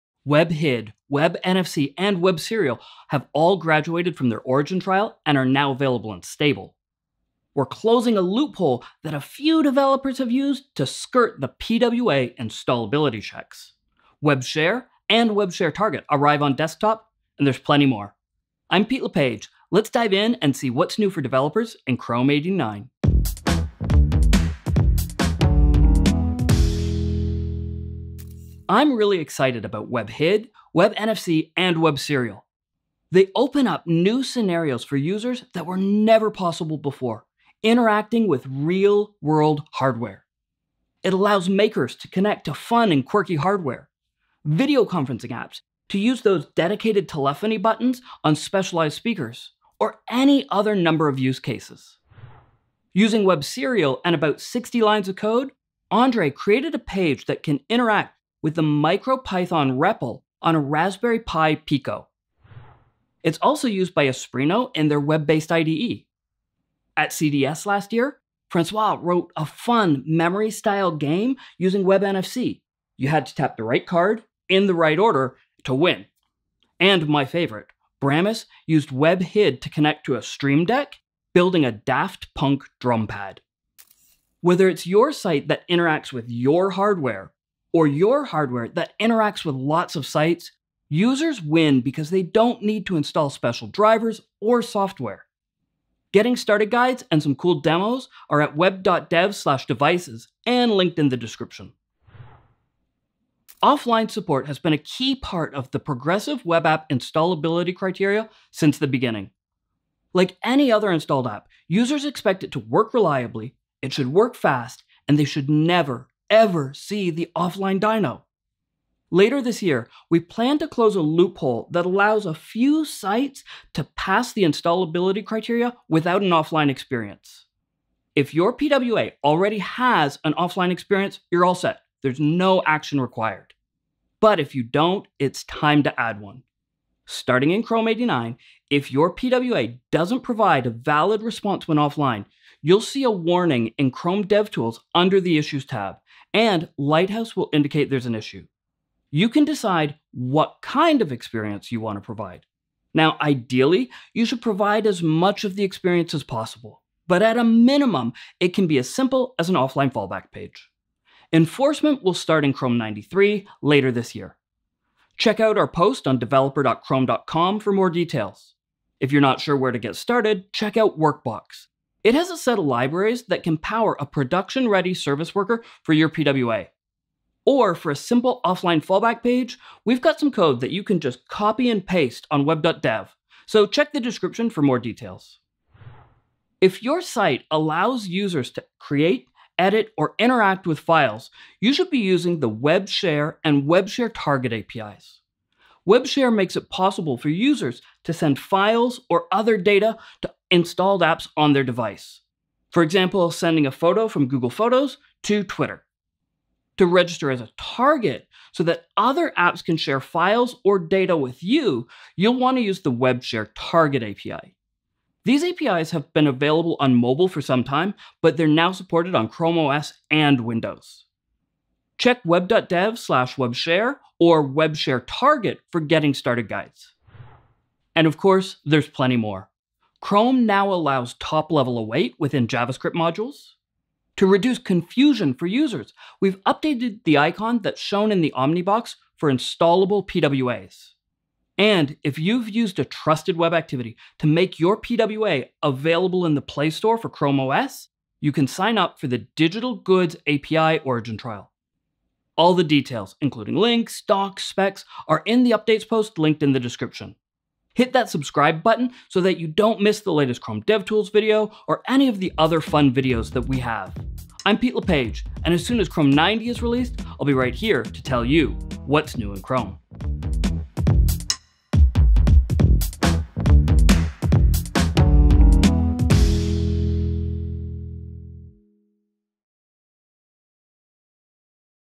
New in Chrome 89: Web Serial, HID, NFC, PWA installability changes, and more!
Chrome 89 is rolling out now! Web HID, Web NFC, and Web Serial have graduated from their origin trials and are now available in stable. We’re closing a loop-hole a few developers used to skirt the PWA installability checks. Web Share, and Web Share Target arrive on the desktop. And there’s plenty more.

I’m Pete LePage, let’s dive in and see what’s new for developers in Chrome 89!

for all of the details including links to docs, specs and samples.

Photo of the Raspberry Pis and Arduino by Harrison Broadbent on Unsplash